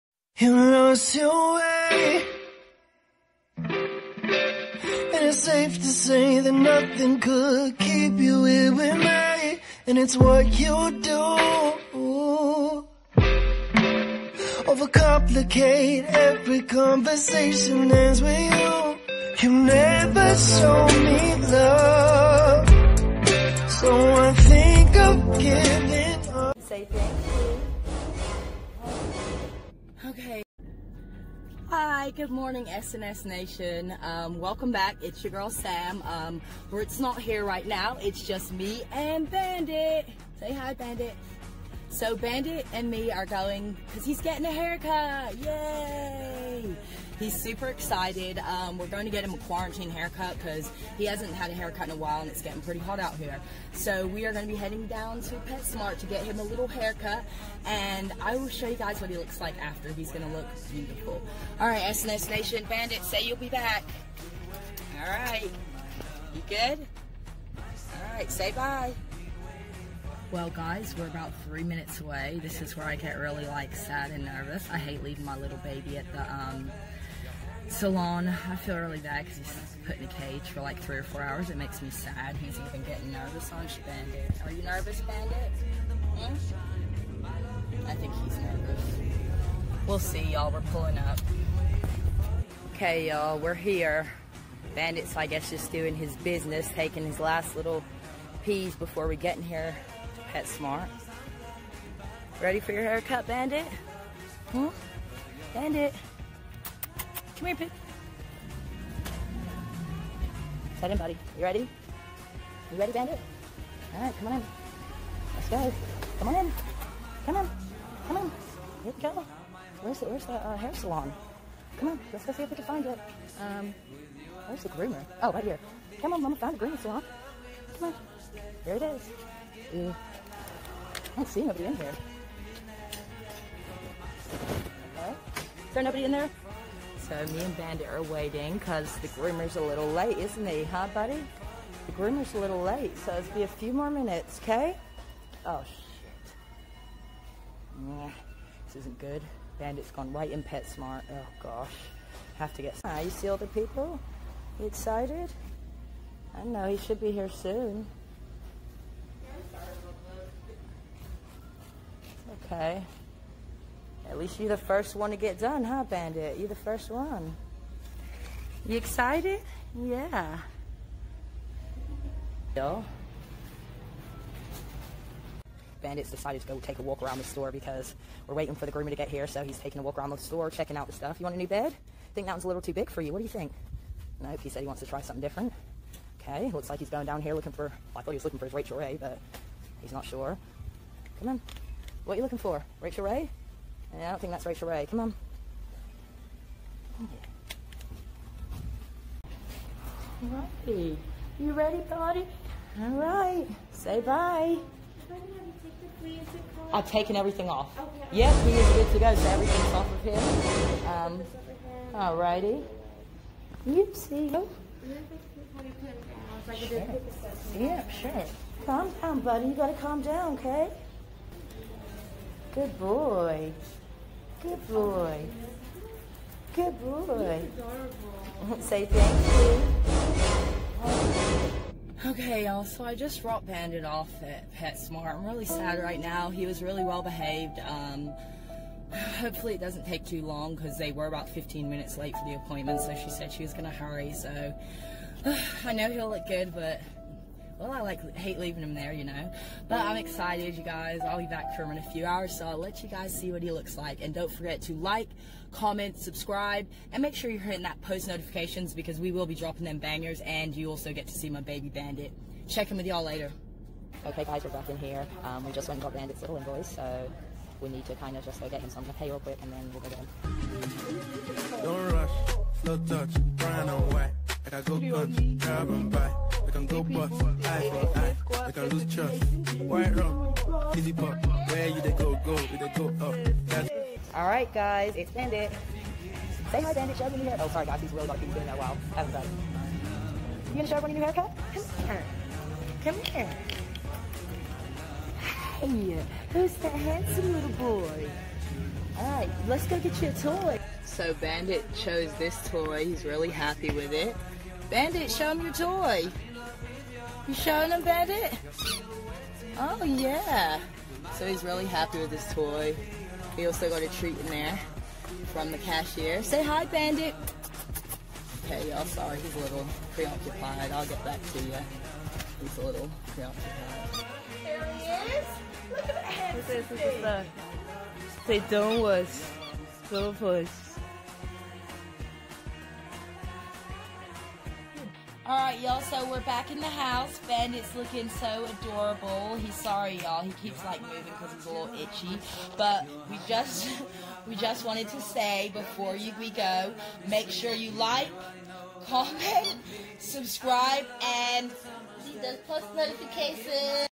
A Haircut Day for a Dog | Sam and Britli Airtight
Sam loves him oso much

We Love you S & S SQUAD